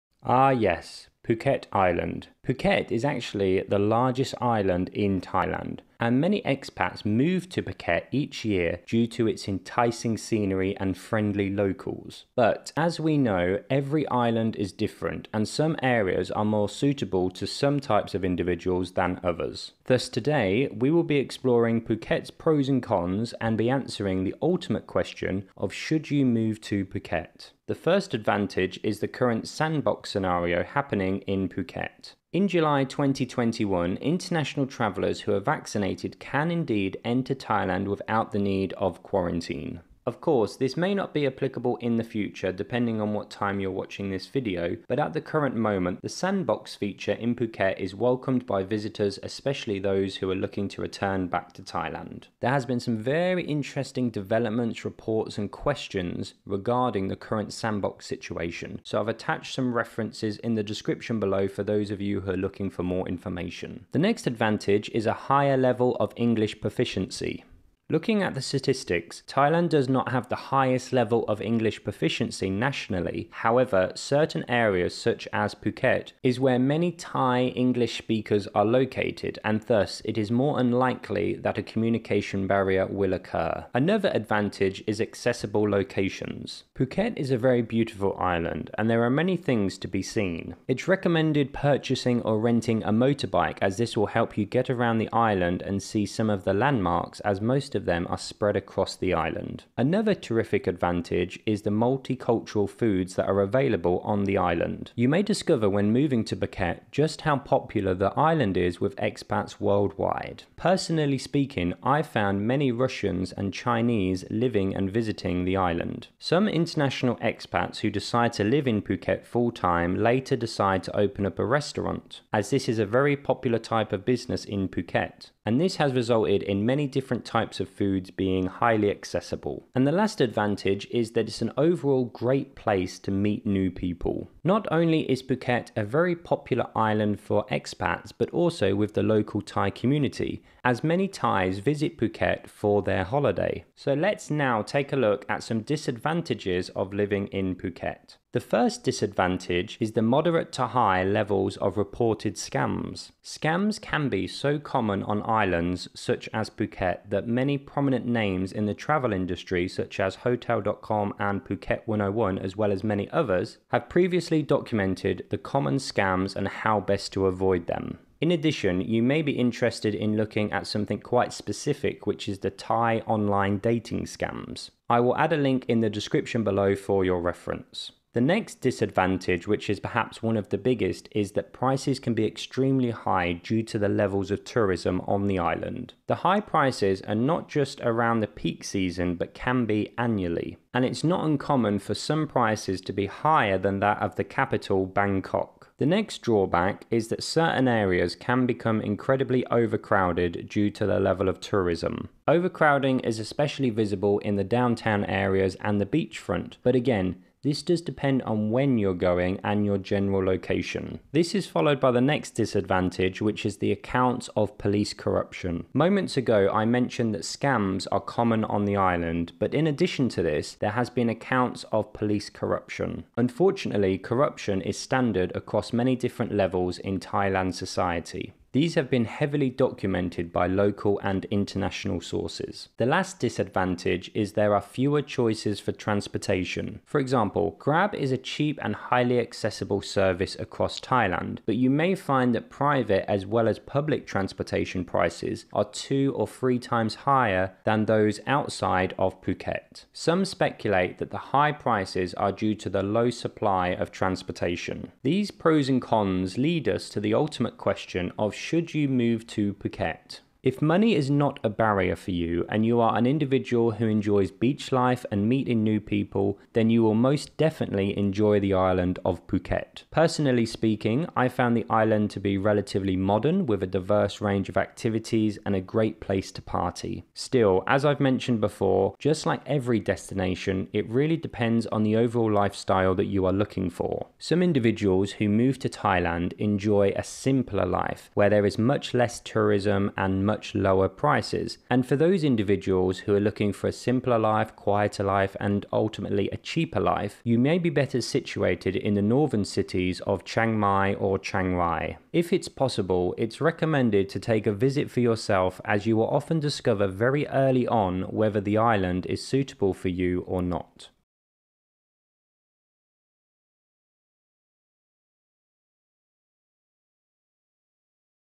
Should you move to Phuket Thailand? - Phuket Pros And Cons - Moving TO Phuket
Due to the current Covid situation. Not all pros and cons of Phuket may be applicable (at this moment).

Phuket is officially the largest island in Thailand, and many expats move to Phuket each year due to its enticing scenery and friendly locals. But every island is different, and some areas are more suitable for some individuals than others. Thus today, we will be exploring Phuket pros and cons and be answering the ultimate question of should you move to Phuket Thailand?

Pros Of Living In Phuket Thailand
Current Sandbox
In July 2021, international travelers who are vaccinated can enter Thailand without the need for quarantine. Of course, this may not be applicable in the future, but at the current moment, the sandbox feature in Phuket is welcomed by visitors, especially those returning to Thailand.

There have been some interesting developments, reports, and questions regarding the current sandbox situation. Attached above are some references for those of you who are looking For more information.

Higher Level Of English Proficiency
One great thing about living on a tourist island such as Phuket is the higher level of English proficiency due to the interaction between locals and expats. Statistically speaking, Thailand does not have the highest level of English proficiency nationally; however, certain areas such as Phuket are where many Thai English speakers are located. Thus, it is more unlikely a communication barrier will occur.

Accessible Locations
Phuket is a beautiful island, and it’s the biggest island in Thailand. There are so many things that can be seen! For those looking at Phuket pros and cons because you are moving to the island, it’s recommended to purchase or rent a motorbike as this will help you get around as some landmarks are spread across the island.

Multi-Cultural Foods Available
You may discover when moving to Phuket just how popular the island is with expats worldwide. Personally speaking, I found many Russians and Chinese living and visiting the island. In addition, some international expats who live in Phuket full-time then decide to open restaurants on the island, including some delicious food from their home country.

Great Place To Meet New People
When looking at Phuket pros and cons, one significant advantage is that it’s straightforward to meet all kinds of people as the island is popular with locals all across Thailand. In other words, many Thais visit Phuket for holidays. In addition, many singletons use dating apps to find Thai singles, but a simple walk on the beach can be all you need to meet some great locals.

Cons Of Living In Phuket Thailand
Moderate To High Levels Of Reported Scams
Scammers are so common on islands such as Phuket that many prominent names in the travel industry, such as hotel.com, Phuket101, and others, have previously documented common scams and how best to avoid them. In addition, you may be interested in looking at some of the most common online dating scams.

Prices Can Be Higher Due To The Level Of Tourism
Phuket pros and cons list would not be complete without mentioning one considerable disadvantage, and that is the overall price. Due to the high levels of tourism, prices can be extremely high, especially during peak season. As a result, it’s not uncommon for some prices to be higher than the capital Bangkok!

Should You Move To Phuket Thailand?